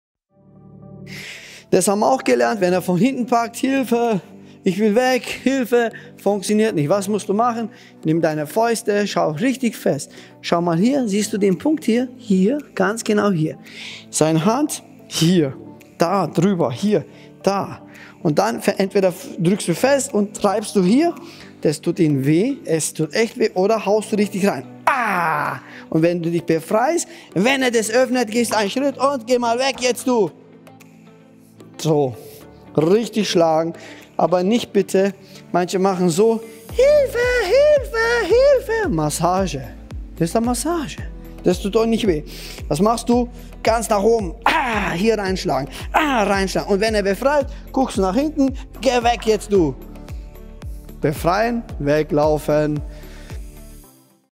KRAV MAGA KIDS TRAINING | Selbstverteidigung gegen Packen von Hinten
Lasst uns gerne Feedback oder Wünsche für kommende Videos da!

OYAKATA
🏠 Hindenburgstr. 50| 85057 Ingolstadt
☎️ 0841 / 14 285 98
📸 oyakatakampfsportakademie